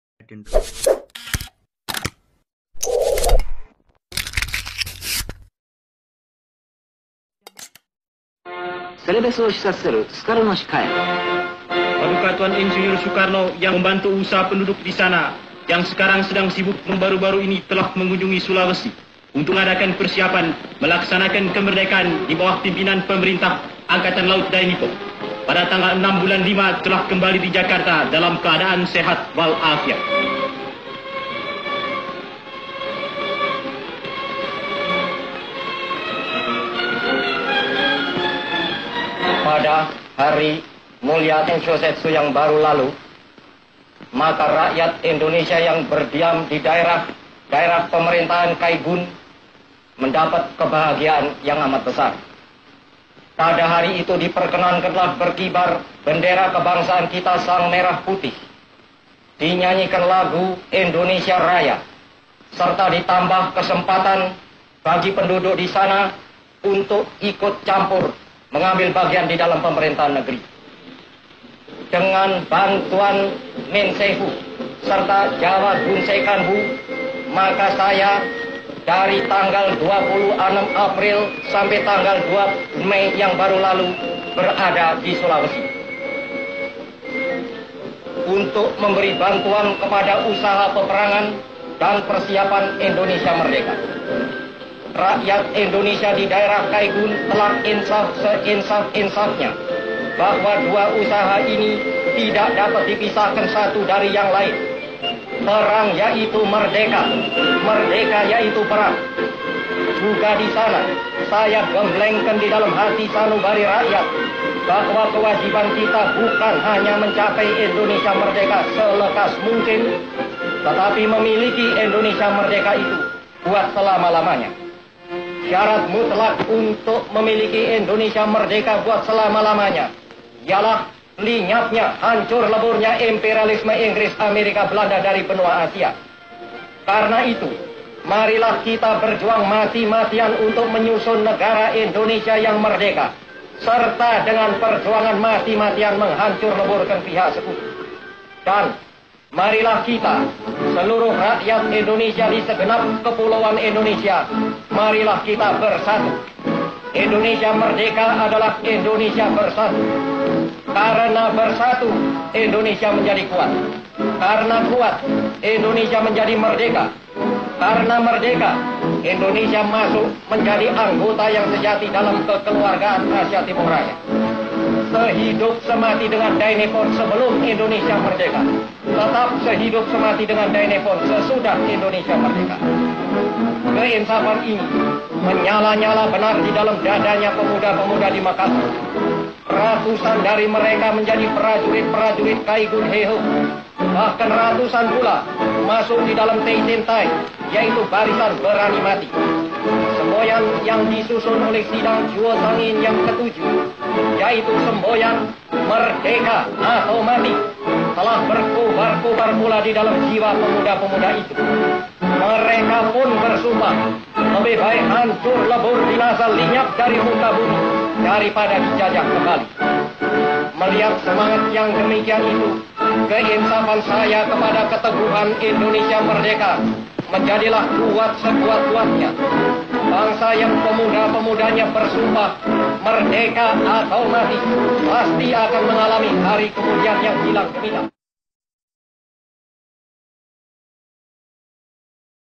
Pidato dan saat P.T.Ir. soekarno kembali di sulawesi lagi
Klo kita amati beberapa video kemerdekaan RI, jepang mendukung kemerdekaan RI. terutama ketika sekutu datang ke indonesia. Konon jepang selalu mensuplai senjata untuk pejuang kita saat melawan sekutu.
Hanya di jaman orba semua kisah lenyap
Sejarah indonesia
Kehidupan lampau
Disini saya hanya mengshere agar generasi saling mengetahui tentang sejarah negara RI.